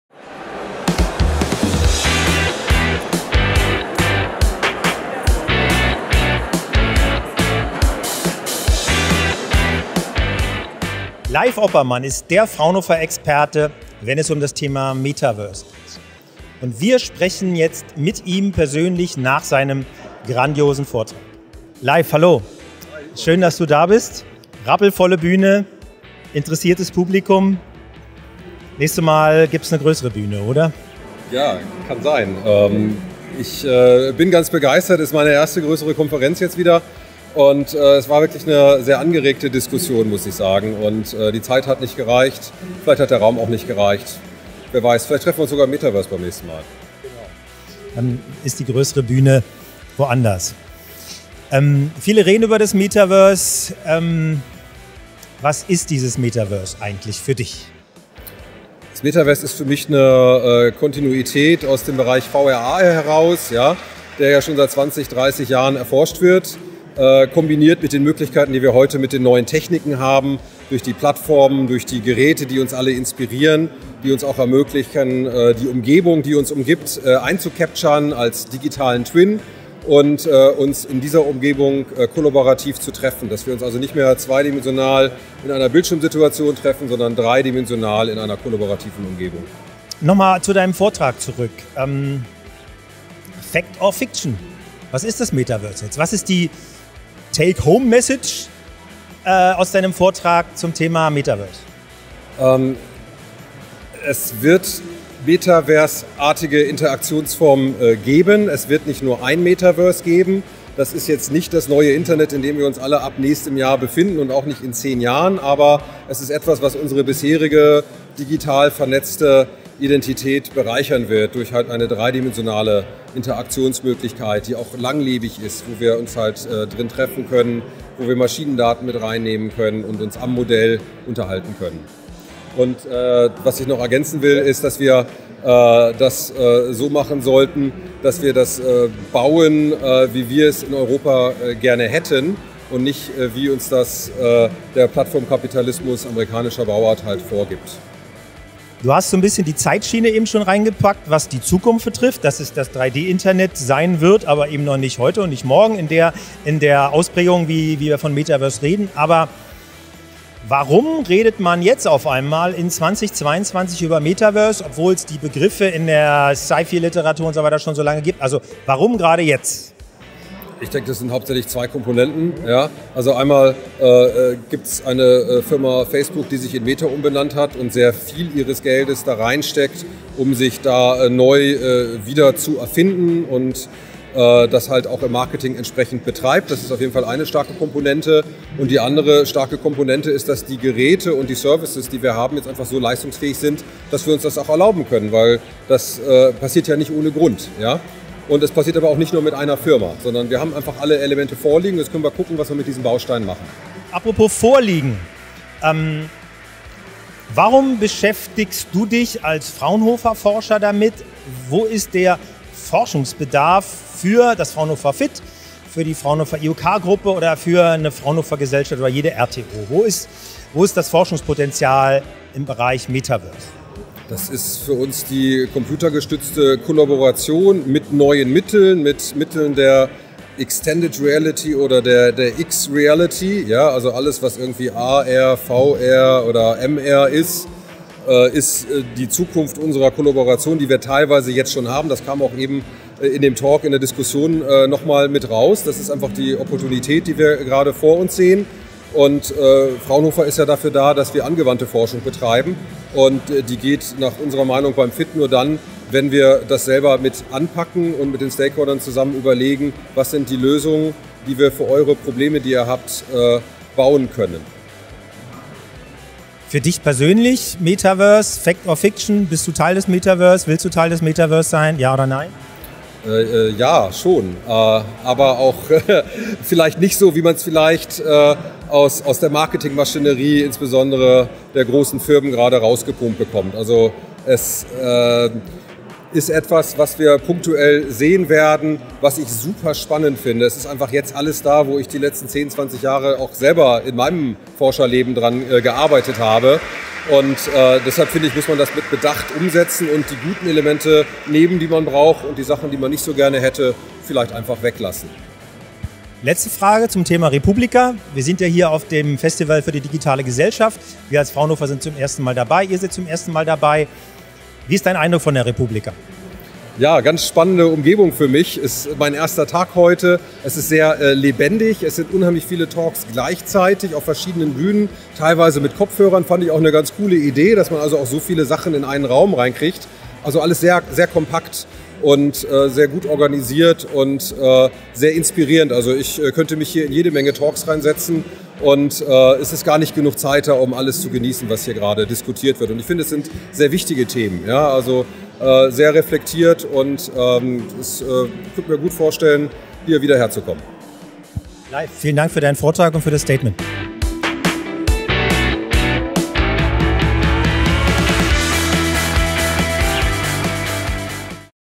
Metaverse: Fact or Fiction? – Dr. Leif Oppermann im Interview auf der re:publica 22
Die re:publica kommt einem Gipfeltreffen der Digitalisierung gleich. Da darf natürlich auch das Metaverse nicht zu kurz kommen. Führend in dessen Forschung steht Dr. Leif Oppermann vom Fraunhofer-Institut für Angewandte Informationssysteme FIT.
Im Interview mit Dr. Dietmar Laß vom Fraunhofer-Verbund IUK-Technologie werden die wichtigsten Standpunkte, Eckdaten und Fakten zum Metaverse aufgezeigt.